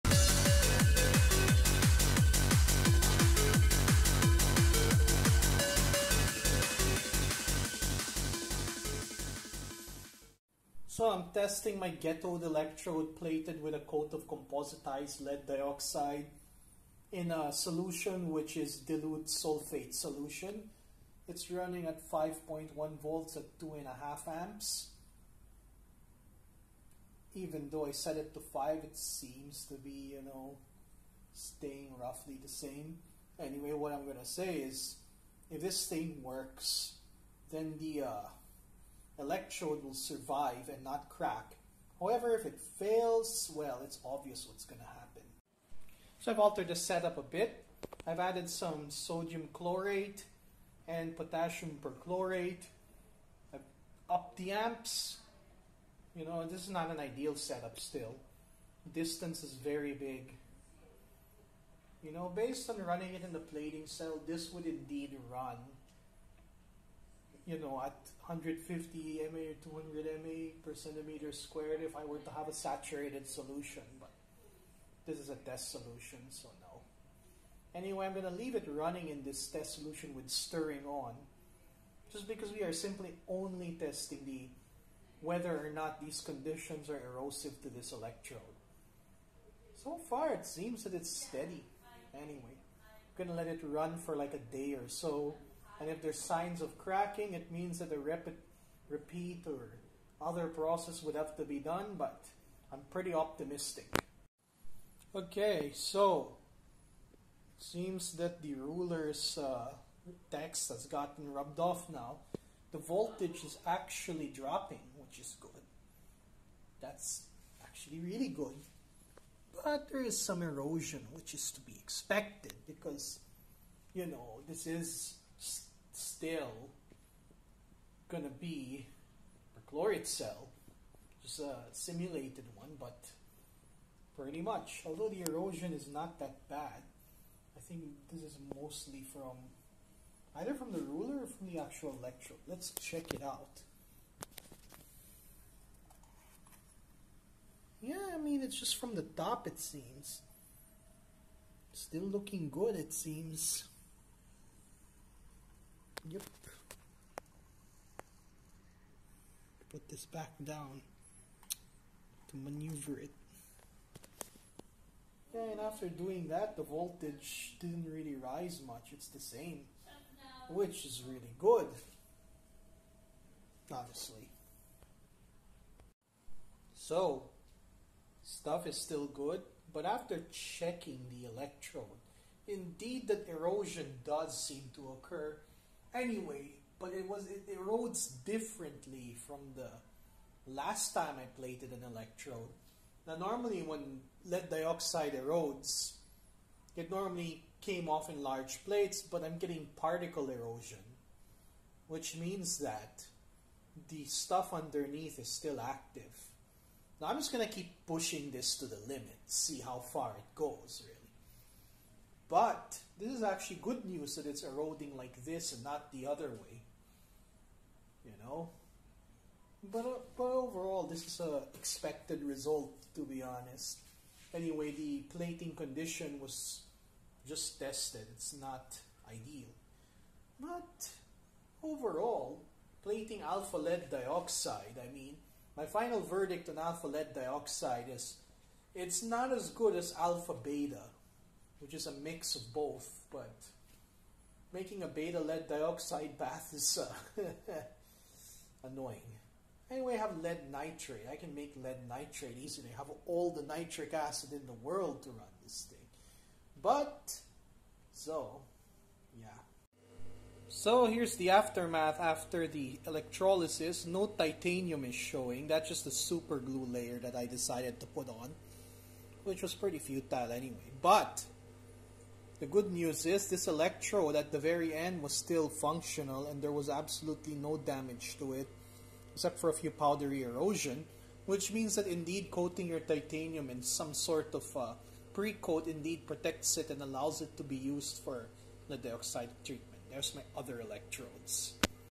"ATO" substrate PbO2
This was the ghetto MMO that I made last time which ive now coated in PbO2.